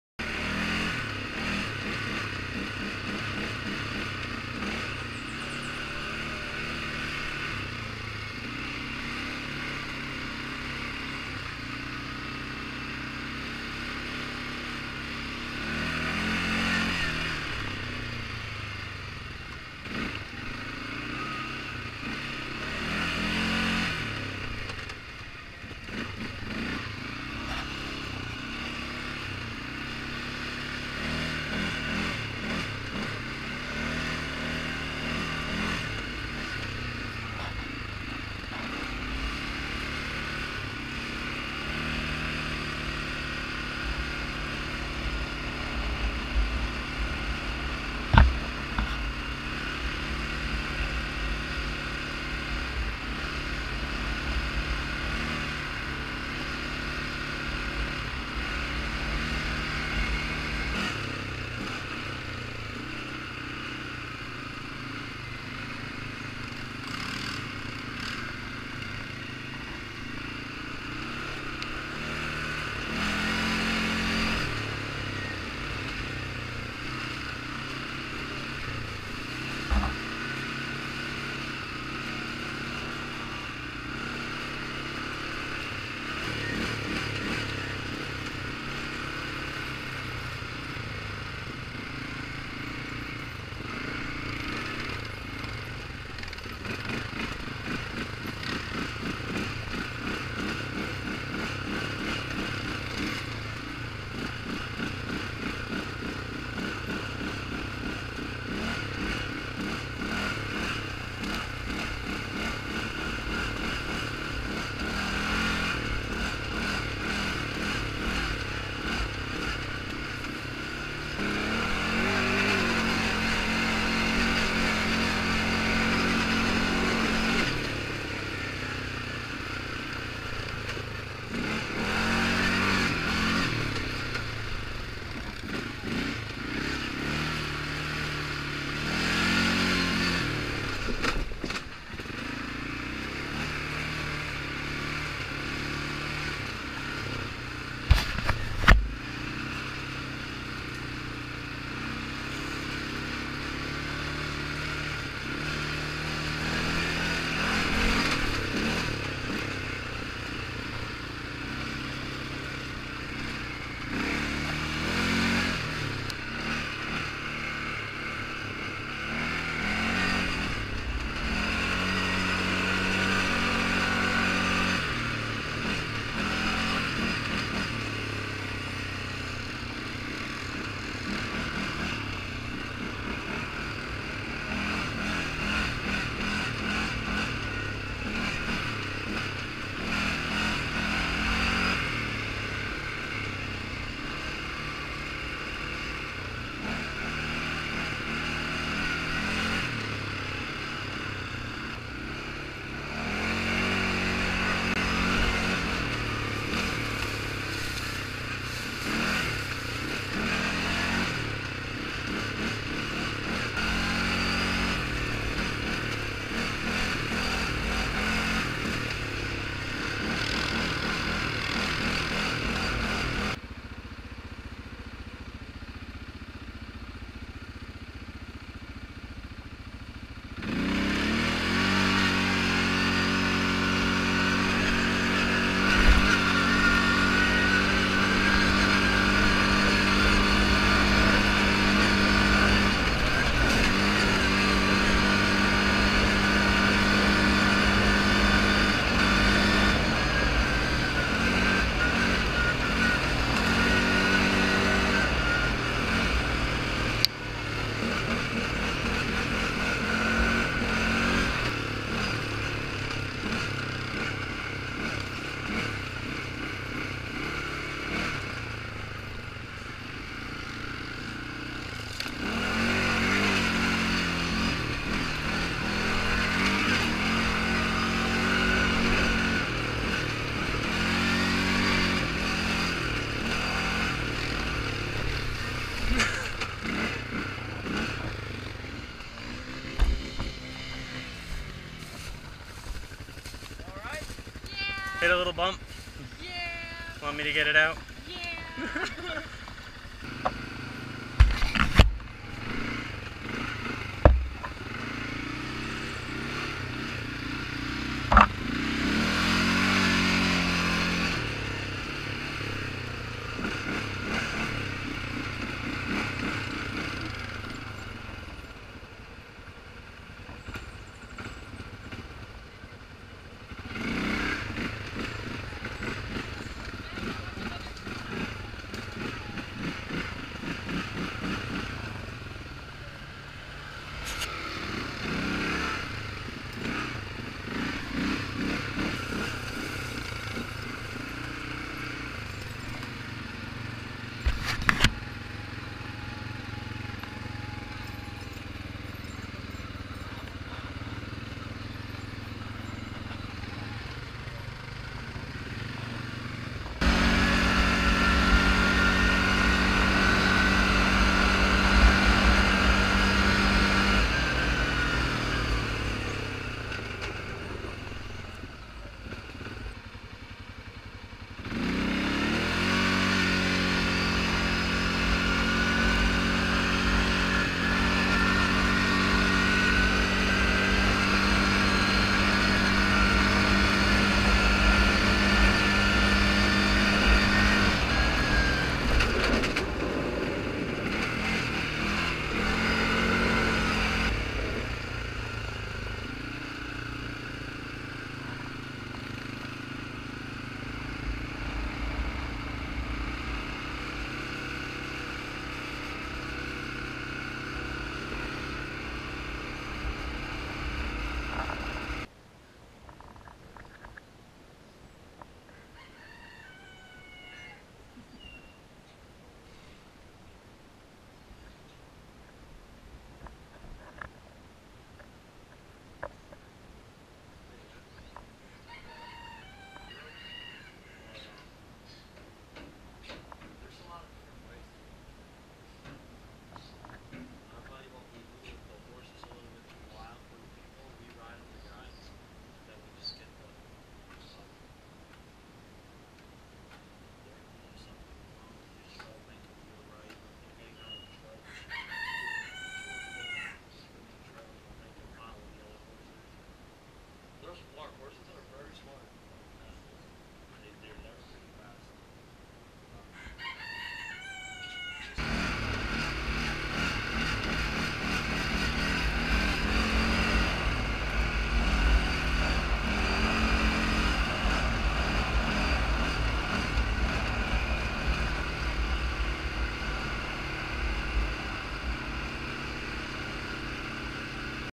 ATV in Puerto Rico - Campo Rico Adventures 2016
Took a trip to Puerto Rico in April,
This guided ATV tour was part of that